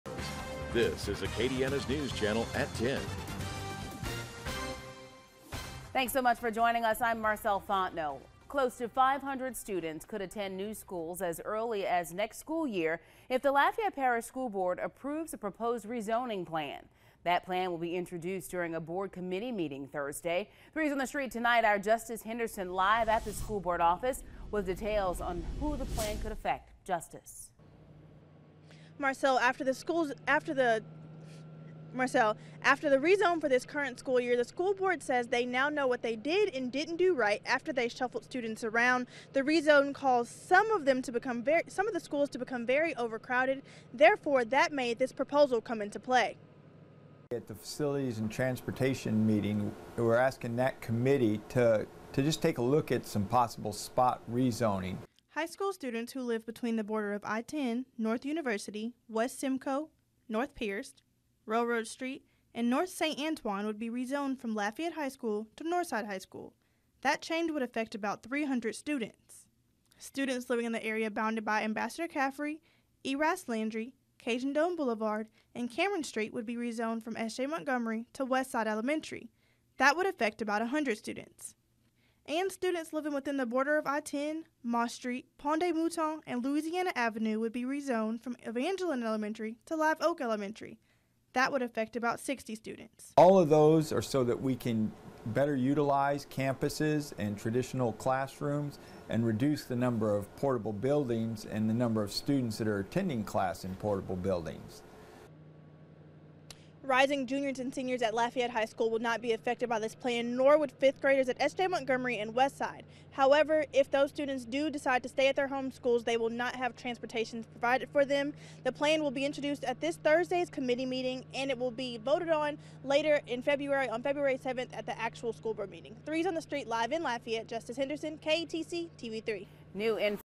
500 students could be rezoned if proposed plan passes
500 students could be rezoned if proposed plan passes